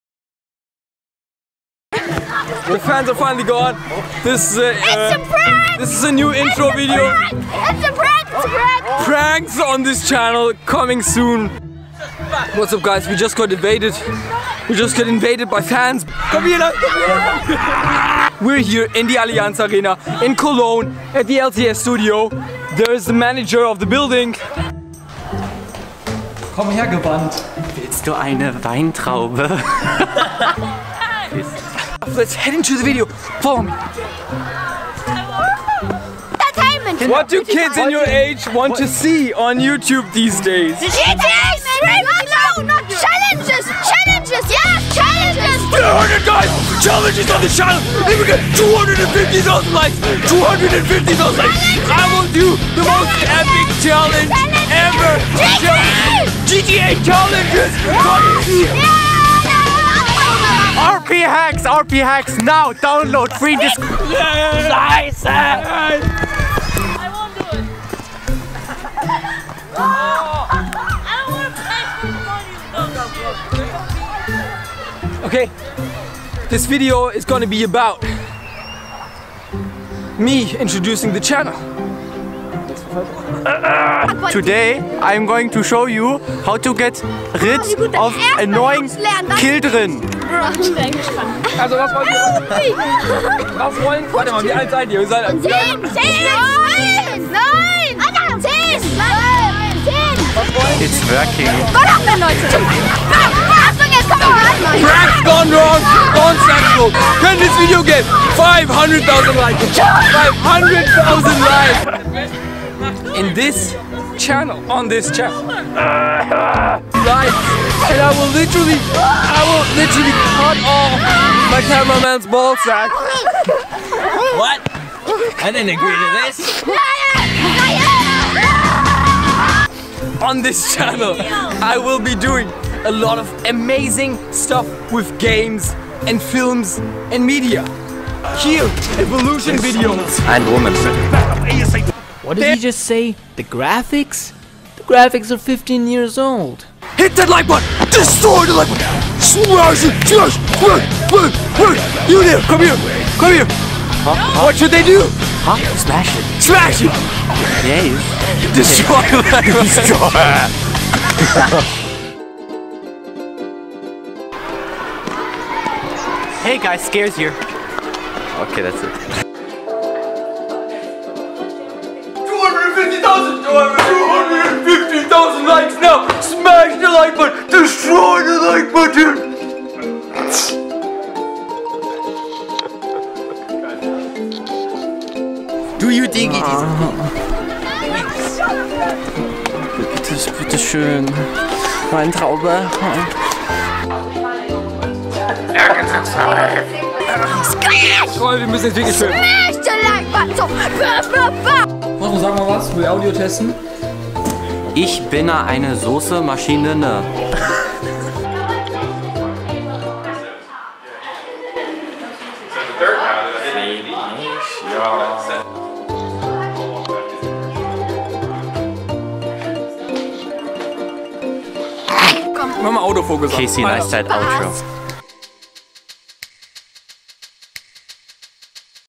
What do Kids want to Watch on Youtube These Days
We are here in the LCS Stadium in cologne, interviewing kids and asking them for their humble opinion about this topic.